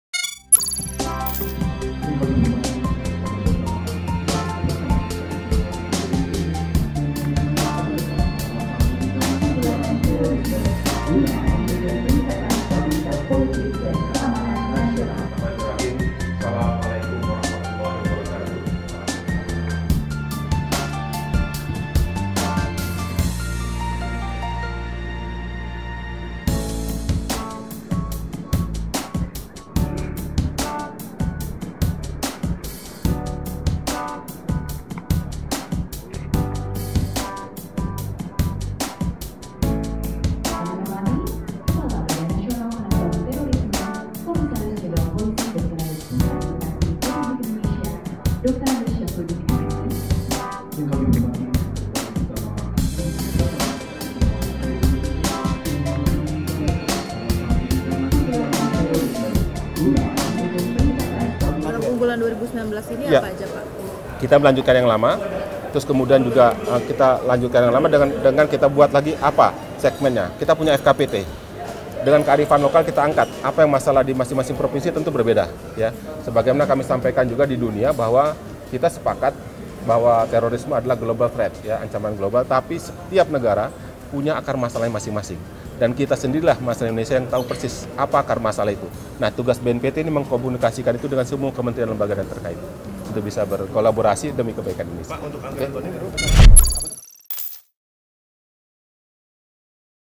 Jurus BNPT Tangkal Radikalisme: Pahami Kearifan Lokal
Badan Nasional Penanggulangan Teroris (BNPT) mengadakan Rapat Kerja Tahunan 2019. kepala BNPT Komjen Suhardi Alius menekankan untuk menangkal terorisme butuh memahami kearifan lokal.